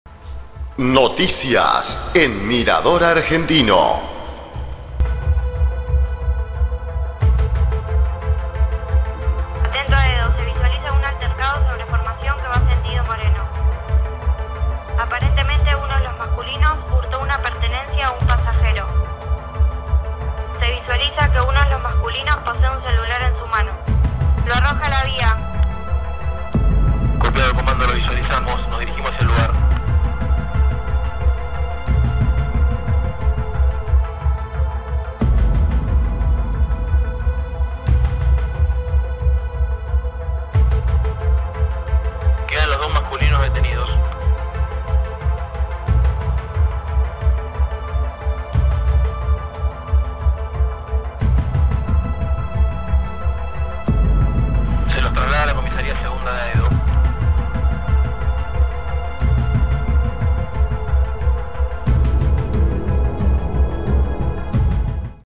HAEDO, NO FALLAN. LOS PUNGAS DE SIEMPRE.
ROBARON UN TELÉFONO CELULAR, COMO EL PASAJERO SE PERCATÓ ARROJARON EL MÓVIL A LAS VÍAS Y FUERON DETENIDOS
 Estación Haedo, línea Sarmiento
 Fecha: 12 de abril
 Hora: 15:30
 Desde las cámaras de seguridad se visualizó como un par de hombres se subieron a un coche del tren que estaba detenido en el andén con el objetivo de robar desde la puerta a quienes viajan.
A los pocos segundos, salieron de la formación y comenzaron una discusión con un pasajero.
 En ese momento, uno de los ladrones arrojó un teléfono móvil por el espacio que hay entre la formación y el andén hacia el tendido de vías.
 Tras ver lo sucedido, desde las cámaras se dio intervención a las fuerzas de seguridad, quienes detuvieron a ambos delincuentes,
 El teléfono, que era del pasajero, fue recuperado y los dos sujetos que habían cometido el hurto fueron trasladados a la comisaría 2da. de Haedo.